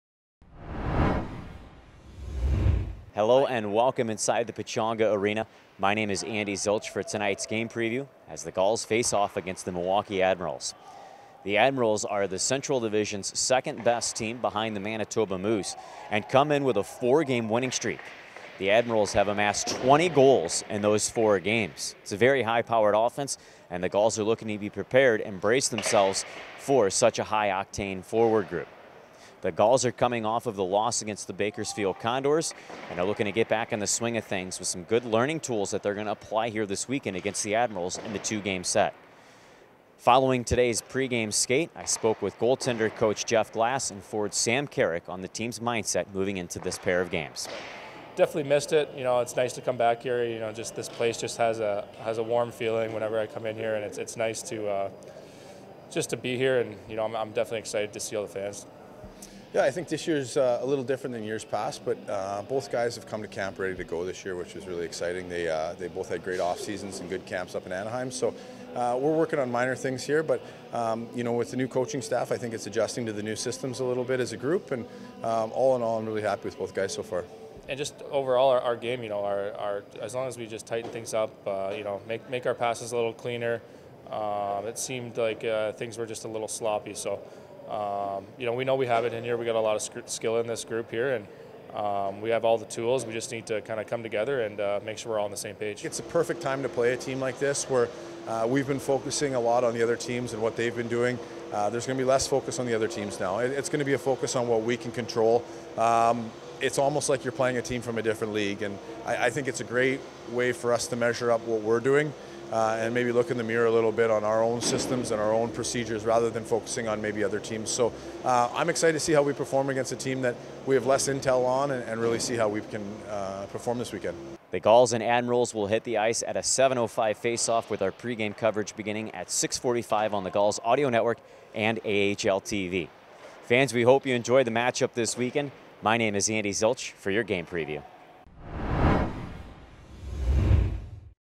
Game Preview - 11.4 vs. Milwaukee
Andy Zilch spoke with goaltending coach Jeff Glass and Sam Carrick ahead of tonight's game against the Admirals.